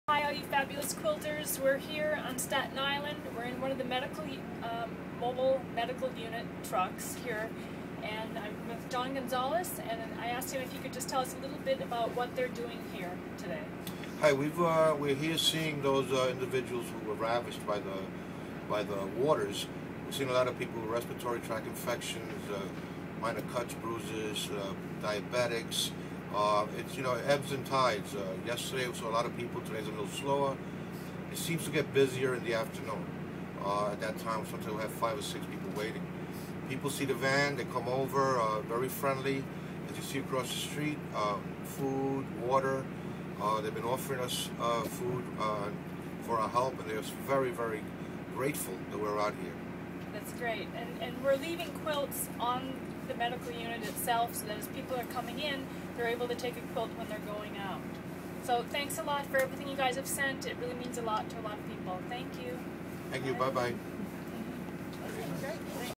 BumbleBeansBASICS: Staten Island Shelter& Mobile Medical unit
Today I went out with Lane from Acacia Network and dropped off quilts for "Sandy" families at a Shelter on Staten Island(Mt. Manresa), and visited one of the Mobile Medical trucks of Acacia Network, to leave quilts with them. As people are getting treatment, they are also able to get a quilt. Here, John Gonzalez, explains a bit about what they do there, how it works.  Thanks for the quilts! You guys are awesome!